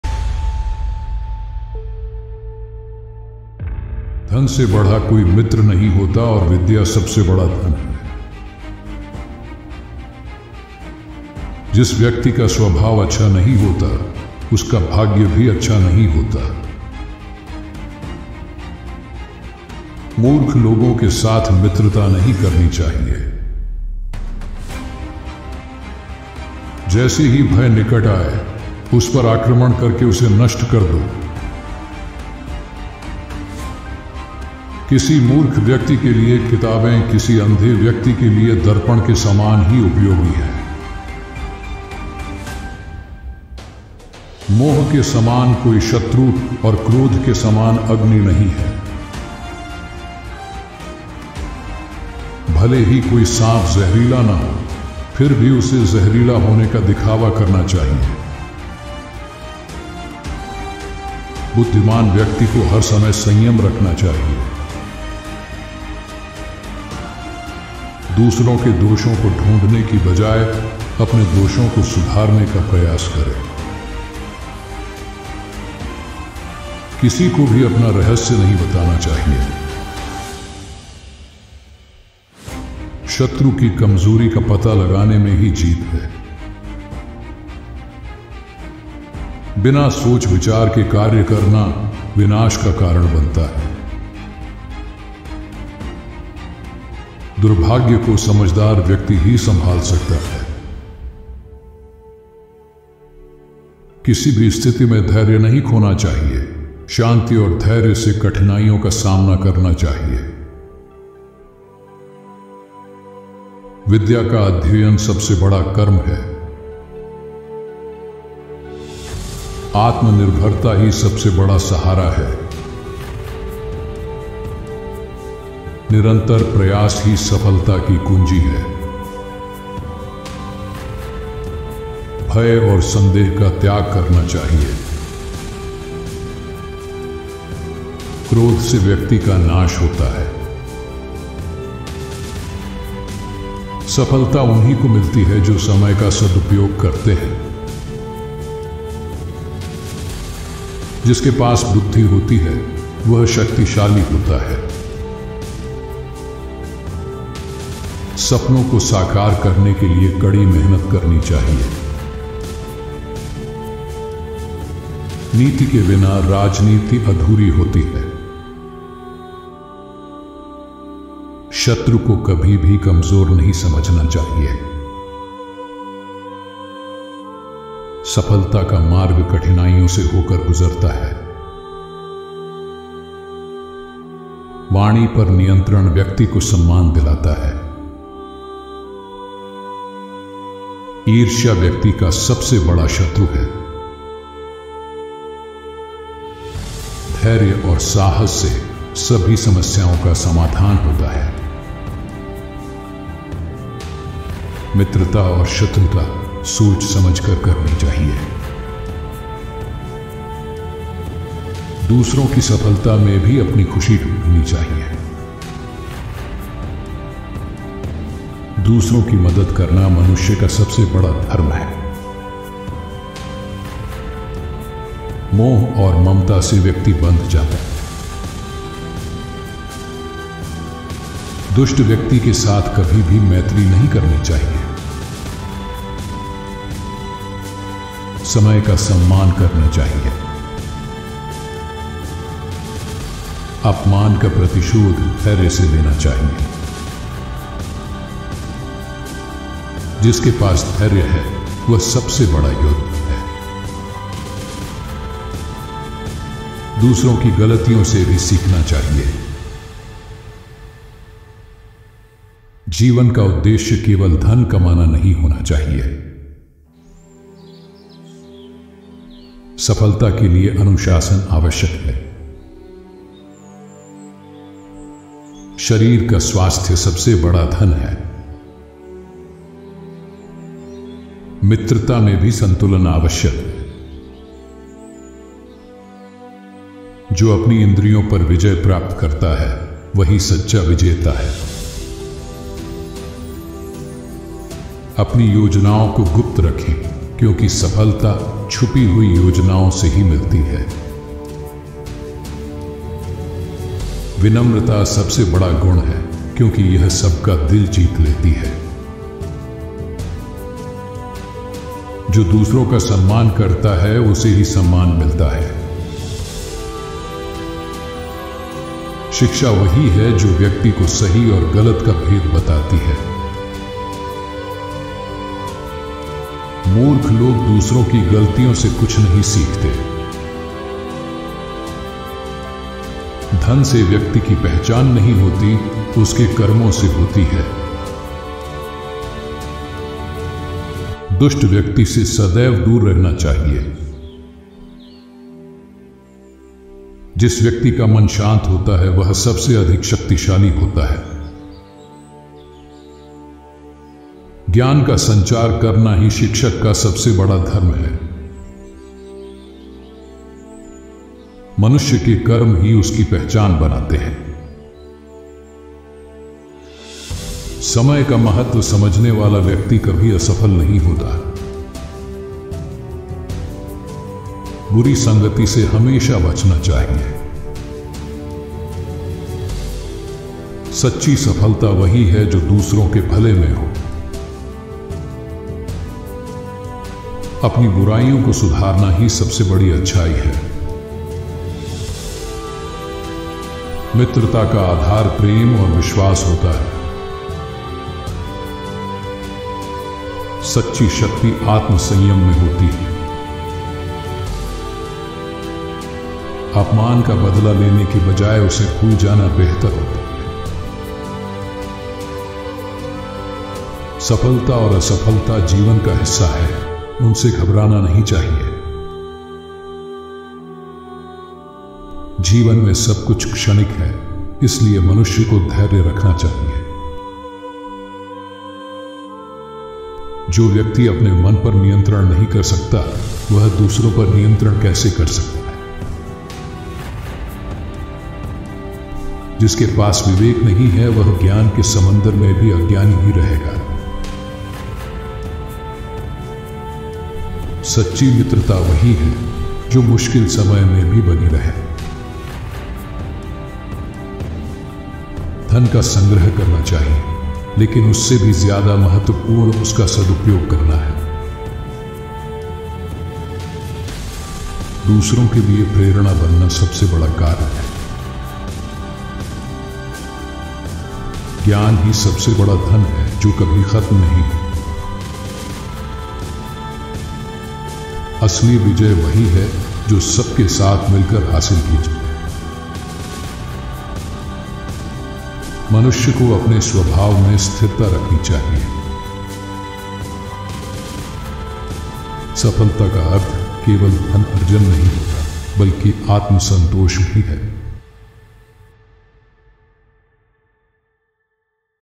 आचार्य चाणक्य के दिल में आग भर देने वाले विचार | Chanakya's Most Powerful Quotes in Hindi
Title: आचार्य चाणक्य के दिल में आग भर देने वाले विचार | Chanakya's Most Powerful Quotes in Hindi

Description:
"Discover the timeless wisdom of Acharya Chanakya through his most powerful and motivational quotes in Hindi. Chanakya, also known as Kautilya, was a great teacher, philosopher, and strategist whose teachings have inspired countless people. In this video, we explore some life-changing quotes from Chanakya Niti that offer guidance on success, leadership, and personal growth.

These quotes are not just words but powerful principles that can transform your life. Whether you're seeking motivation, wisdom, or a deeper understanding of life's challenges, Chanakya's teachings provide valuable insights that are relevant even today.

Welcome to Fire Of Quotes

Disclamimer- This video is for education purposes only